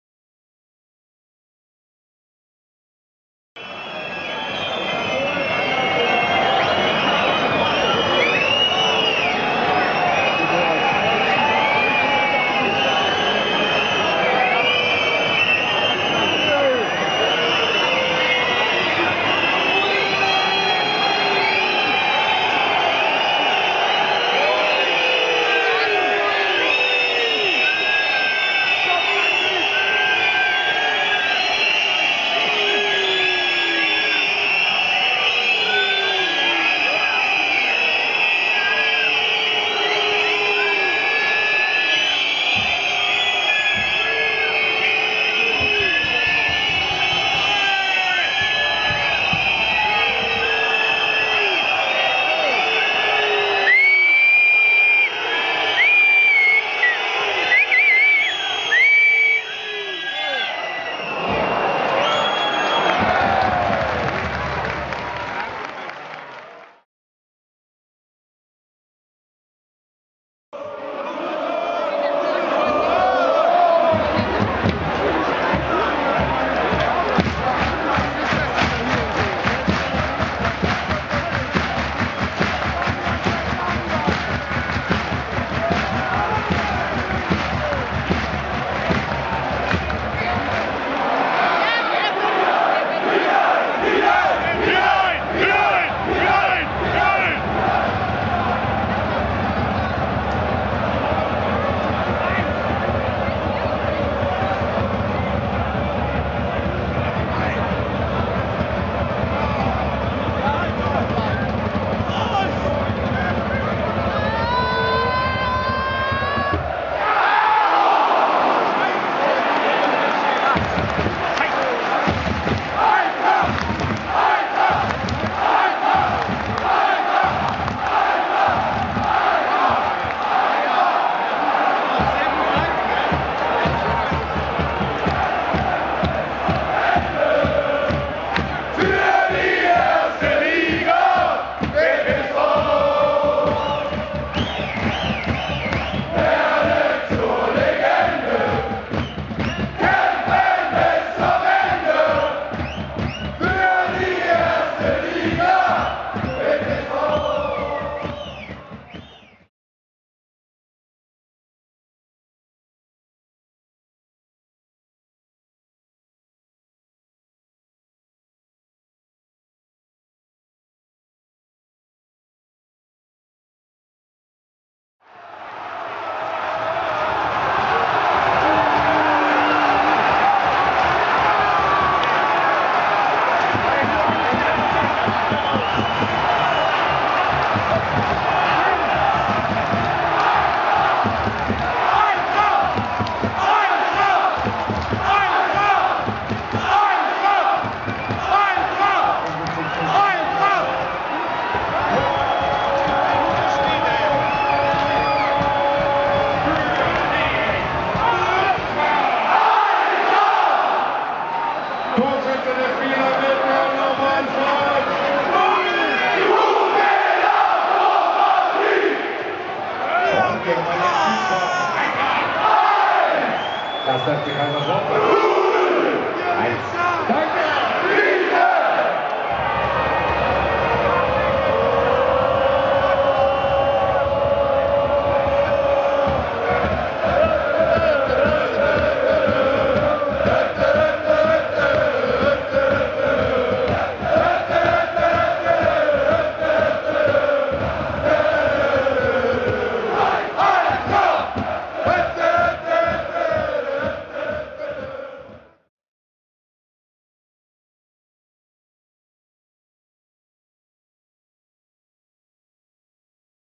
Eintracht Braunschweig vs 1.FC Kaiserslautern - Saison 2012/2013 - Impressionen
Hier ein Kurzvideo zum 2.Ligaspiel zwischen Eintracht Braunschweig und Kaiserslautern. Gefilmt wie immer aus Block 8.

Tore:
0:1 Mitchell Weiser (44.)
1:1 Domi Kumbela (76.)

Zuschauer 20.630